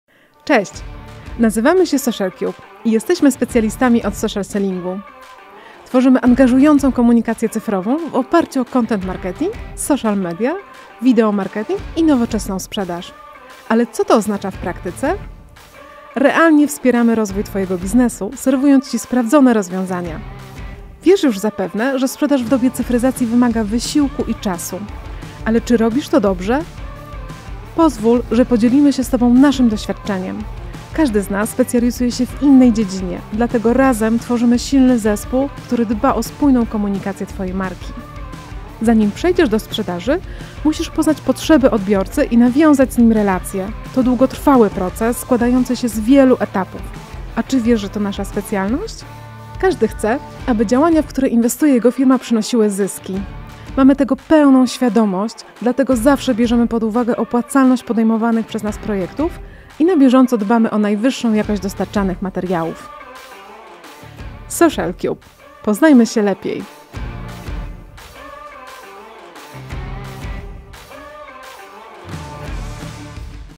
SocialCube: Poznajmy się lepiej
Cześć, nazywamy się SocialCube i jesteśmy specjalistami od social sellingu.

Tworzymy angażującą komunikację cyfrową w oparciu o content marketing, social media, video marketing i nowoczesną sprzedaż.

Poznajmy się lepiej!